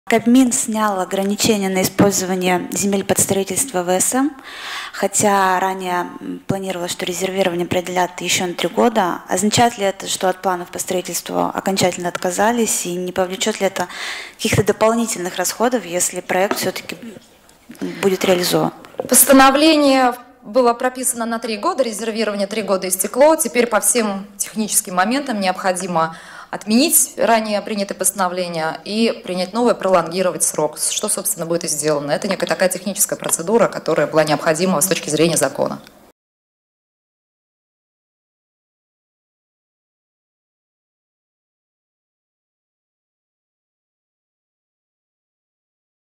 В казанском Кремле объяснили, почему сняты ограничения на земли под ВСМ
В казанском Кремле объяснили, почему правительство Татарстана сняло ограничения на использование земельных участков, зарезервированных под строительство высокоскоростной железнодорожной магистрали Москва — Казань.